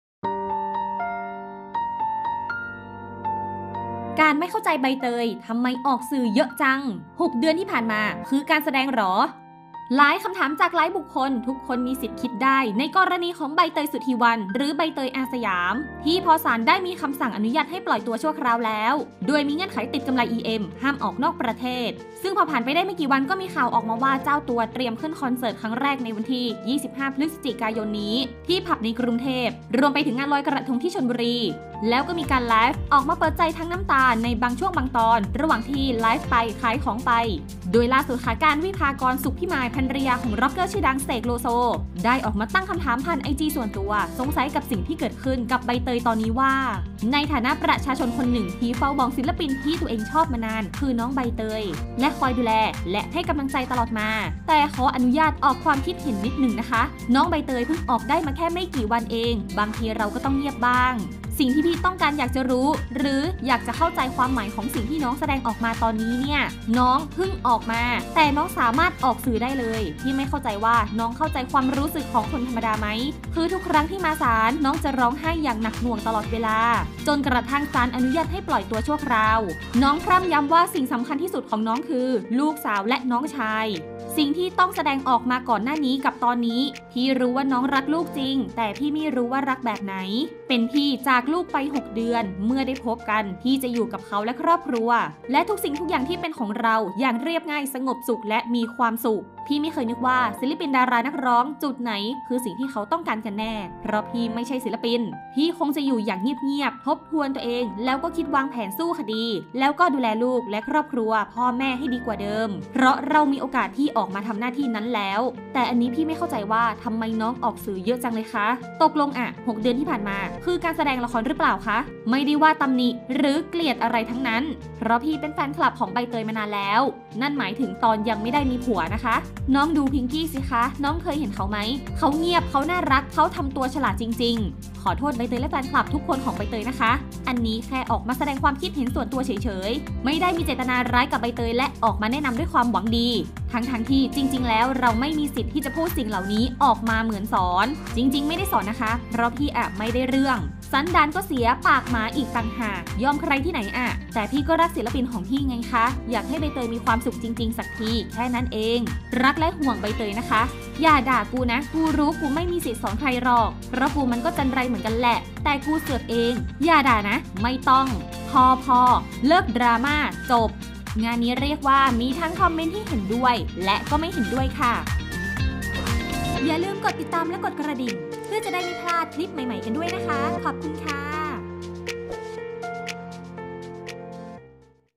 “กานต์” สุดทน ไม่เข้าใจ “ใบเตย” ทำไมออกสื่อเยอะจัง! 6 เดือนที่ผ่านมาคือการแสดงเหรอ?
ได้รับอิสระอีกครั้งสำหรับ "ใบเตย-สุธีวัน กุญชร" ที่ได้รับการปล่อยตัวชั่วคราว และเธอก็มีงานเข้ารัวๆ เดินหน้าทำงานต่อทันที ล่าสุดทางด้าน "กานต์ วิภากร" ภรรยา "เสก โลโซ" ได้ออกมาเคลื่อนไหวโพสต์อินสตราแกรมส่วนตัว wiphakorn15 ตั้งข้อสงสัยเรื่องใบเตย โดยบอกว่า

"ในฐานะประชาชนคนหนึ่งที่คอยเฝ้ามองศิลปินที่ตัวเองชอบมานานคือน้องใบเตย และคอยดูแลและให้กำลังใจตลอดมา แต่ขออนุญาตออกความคิดเห็นนิดนึงนะคะ น้องใบเตยเพิ่งออกมาได้แค่ไม่กี่วันเอง บางทีเราก็ต้องเงียบบ้าง สิ่งที่พี่ต้องการอยากจะรู้หรืออยากจะเข้าใจความหมายของสิ่งที่น้องแสดงออกมาตอนเนี่ย

- น้องเพิ่งออกมาแต่น้องสามารถออกสือได้เลยพี่ไม่เข้าใจว่าน้องจะเข้าใจความรู้สึกของคนธรรมดาไหม
- คือทุกครั้งที่มาศาลน้องจะร้องไห้อย่างหนักหน่วงตลอดเวลาจนกระทั่งศาลอนุญาตปล่อยตัวชั่วคราว
- น้อง พร่ำย้ำว่าสิ่งสำคัญที่สุดของน้องคือลูกสาวและน้องชาย
- สิ่งที่ต้องแสดงออกมาก่อนหน้านี้กับตอนนี้พี่รู้ว่าน้องรักลูกจริงแต่พี่ไม่รู้ว่ารักแบบไหน
- เป็นพี่จากลูกไปหกเดือนเมื่อได้พบกันพี่จะอยู่กับเขาและครอบครัวและทุกสิ่งทุกอย่างที่เป็นของเขาเรา อย่างเรียบง่ายสงบสุขและมีความสุข
- พี่ไม่เคยนึกหรอกว่าศิลปินดารานักร้อง จุดไหนคือสิ่งที่เขาต้องการกันแน่เพราะพี่ไม่ใช่ศิลปิน
- พี่คงจะอยู่อย่างเงียบเงียบทบทวนตัวเองแล้วก็คิดวางแผนสู้คดีแล้วก็ดูแลลูกและครอบครัวพ่อแม่ให้ดีกว่าเดิมเพราะเรามีโอกาสที่ออกมาทำหน้าที่นั้นแล้ว

- แต่อันนี้พี่ไม่เข้าใจว่าน้องทำไมออกสื่อเยอะจังเลยค่ะตกลงอ่ะหกเดือนที่ผ่านมาคือการแสดงละครหรือเปล่าคะ
- ไม่ได้ว่าตำหนิหรือเกลียดหรืออะไรทั้งนั้นเพราะพี่เป็นแฟนคลับของใบเตยมานานแล้ว นั่นหมายถึงตอนยังไม่ได้มีผัวนะคะ
- น้องดูพิ้งกี้สิคะน้องเคยเห็นเขาไหมเค้าเงียบเค้าน่ารักเค้าทำตัวเค้าฉลาดจริงๆ
- ขอโทษใบเตยและแฟนขับทุกคนของใบเตยนะคะ อันนี้แค่ออกมาแสดงความคิดเห็นส่วนตัวเฉยเฉย
- ไม่มีเจตนาร้ายกับใบเตยเลยและออกมาแนะนำด้วยความหวังดี
- ทั้งทั้งที่จริงๆแล้วเราไม่มีสิทธิ์ที่จะพูดสิ่งเหล่านี้ออกมาเหมือนสอน
- จริงๆไม่ได้สอนนะคะ เพราะพี่อ่ะก็ไม่ได้เรื่อง สันดานก็เสียปากหมาอีกต่างหาก ยอมใครที่ไหนอ่ะ
- แต่พี่ก็รักศิลปินของพี่ไงคะ อยากให้ใบเตยมีความสุขจริงๆสักทีแค่นั้นเองค่ะ
รักและห่วงใบเตยนะคะ

อย่าด่ากูนะกูรู้กูไม่มีสิทธิ์สอนใครหรอกเพราะกูมันก็จัญไรเหมือนกันแหละ
แต่กูเสือกเอง อย่าด่านะไม่ต้องพอพอ เลิกด่าม่าจบ !!"